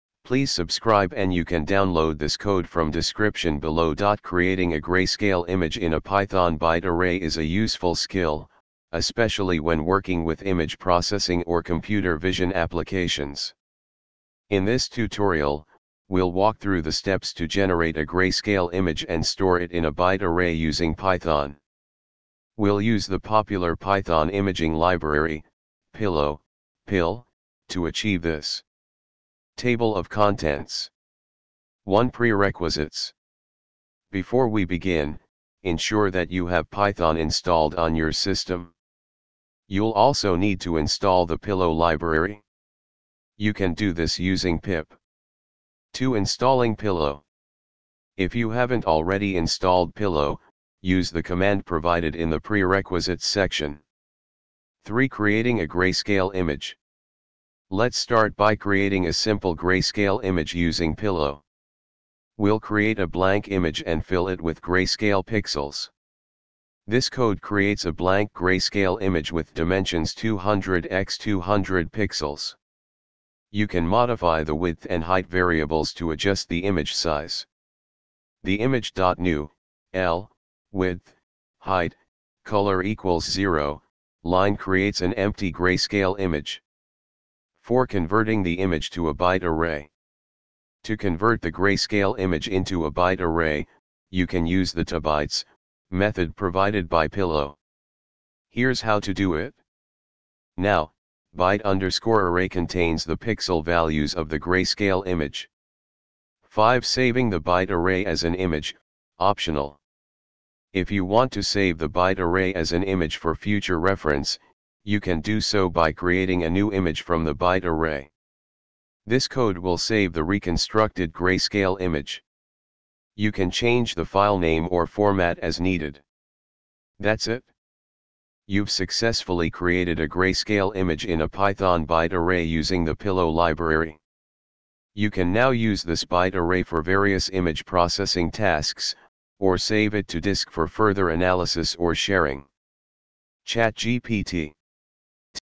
Creating a grayscale image in a python byte array
Grayscale images are images without any color information, represented using shades of gray. In this tutorial, we will walk through the process of creating a grayscale image in Python and storing it in a byte array. We will be using the Python Imaging Library (PIL), also known as the Pillow library, to work with images.
Before you begin, make sure you have Python installed on your system. You can install Pillow, the Python Imaging Library fork, using pip if you haven't already:
In this example, we first create a new grayscale image with a specified width and height using the Image.new method from the Pillow library. We use the 'L' mode, which represents 8-bit pixels in black and white.
We then access the image data as a list of pixels using the load() method. Inside the nested loops, you can set the pixel values based on your desired grayscale logic. In this case, we set all pixels to a constant value of 128, resulting in a medium gray tone.
The image can be saved to a file using the save() method if you want to visualize the output. Finally, the grayscale image is converted to a byte array using the tobytes() method and stored in the image_byte_array variable.
Feel free to modify the pixel values or grayscale logic according to your requirements to create different grayscale effects.
ChatGPT